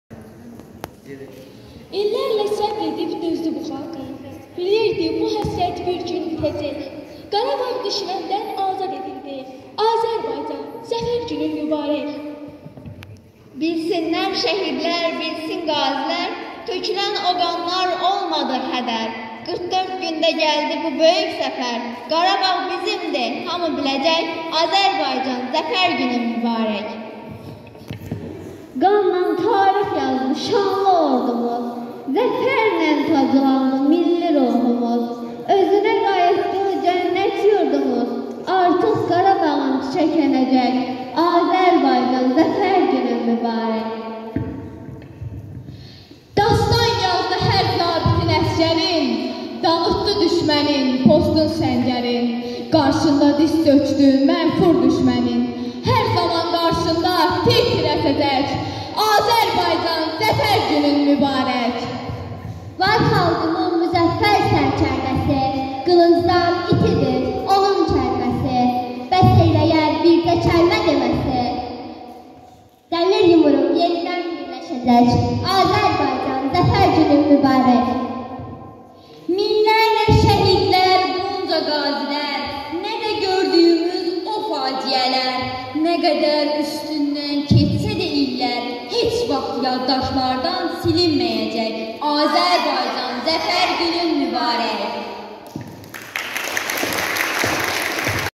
Qanla tarix yazdı şanlı ordumuz Zəfər Günü ilə əlaqədar Riyaziyyat gecəsində şagirdlər şeir söylədi
Qanla tarix yazdı şanlı ordumuz... Zəfər Günü ilə əlaqədar "Riyaziyyat gecəsi"ndə şagirdlər şeir söylədilər